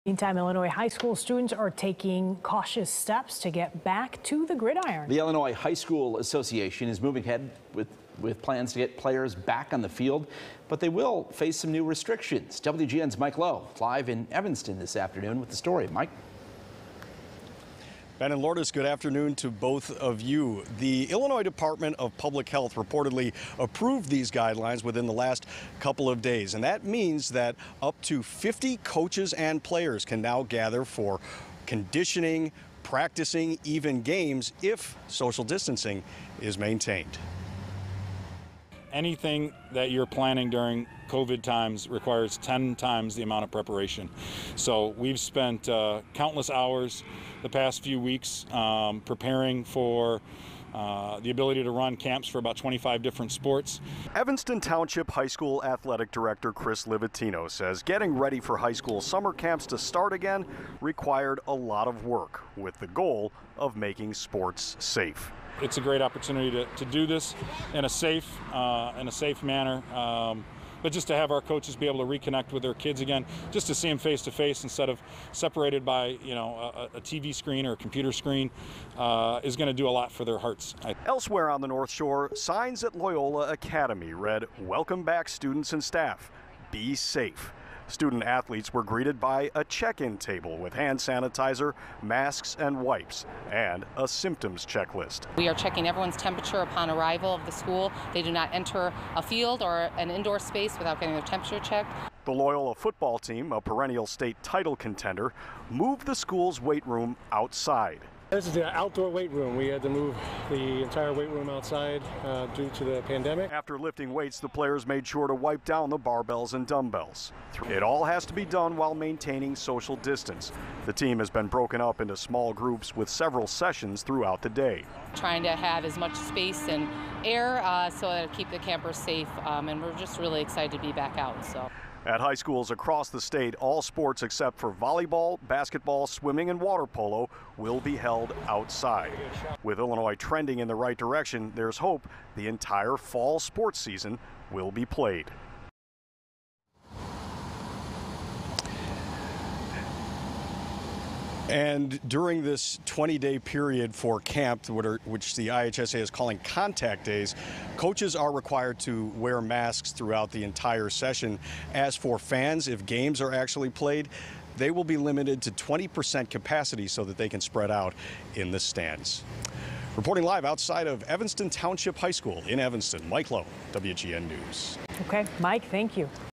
Illinois high school athletes OK'd for next phase of 'Return to Play' coronavirus guidelines
High school athletes are getting ready for their upcoming seasons, now that the Illinois High School Association announced it's ready to move to Phase 4 amid the coronavirus pandemic.